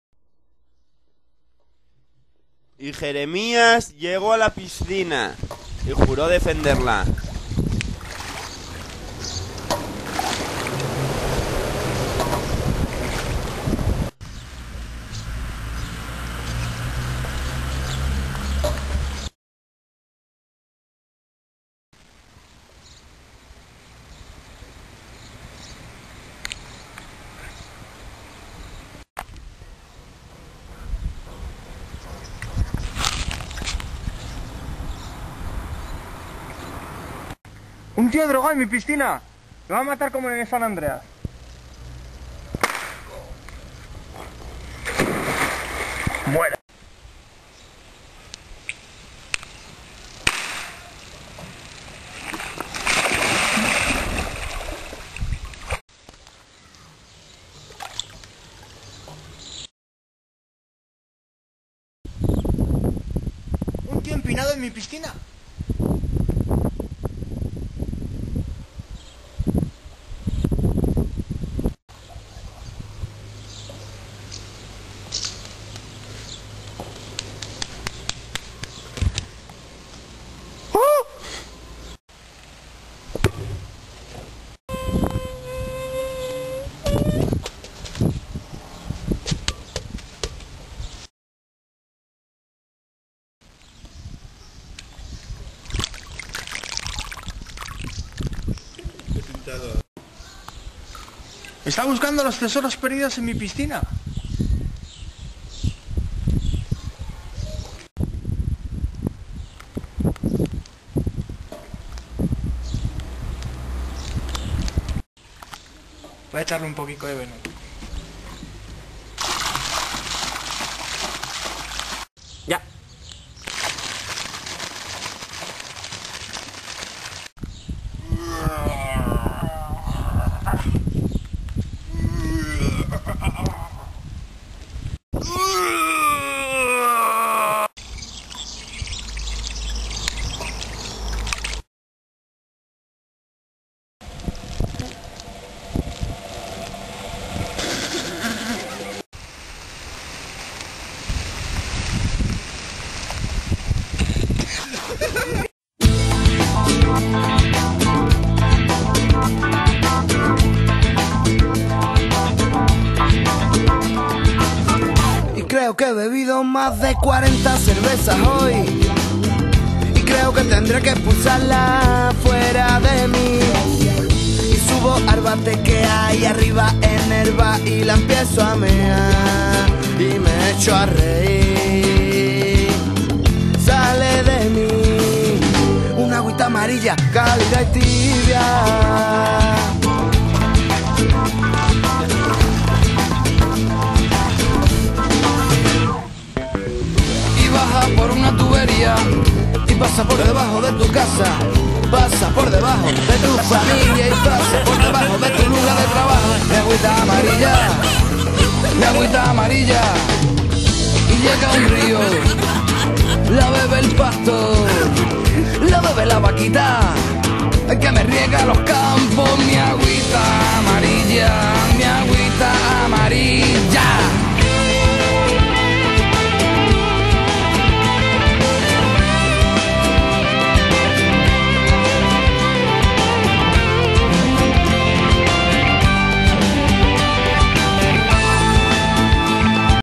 Jeremias
Coro de Jeremias, un individuo que lucha por mantener su piscina limpia de impuros de espiritu.